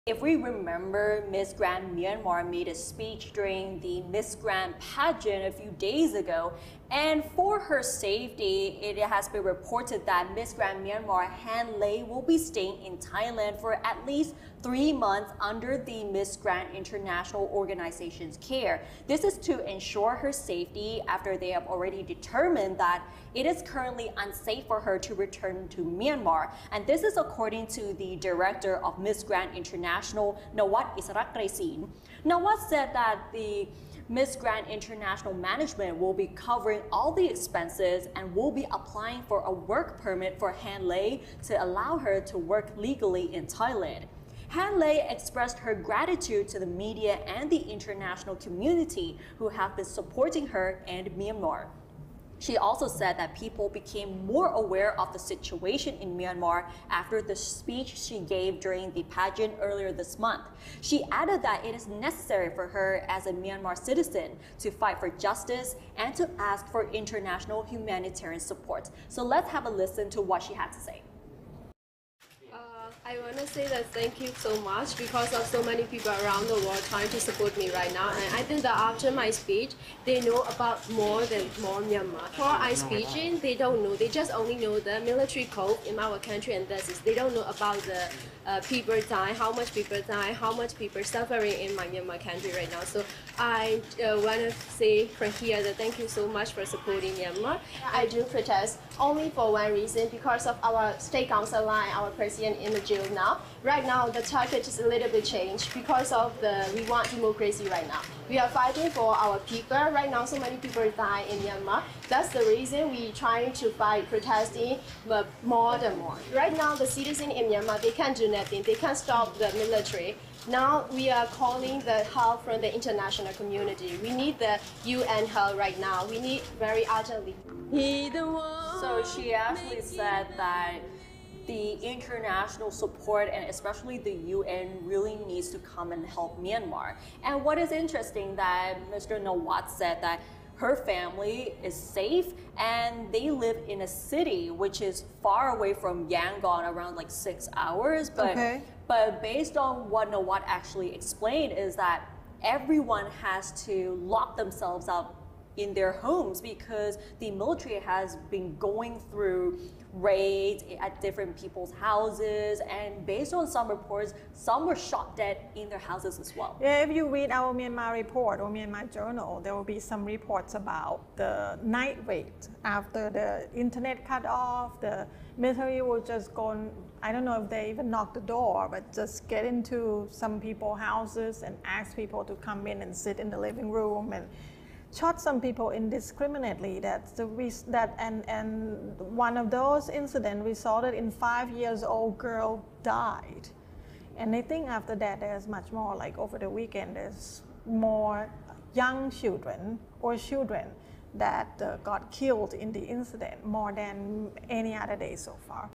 Miss Grand International ensures safety of Miss Grand Myanmar
31st March 2021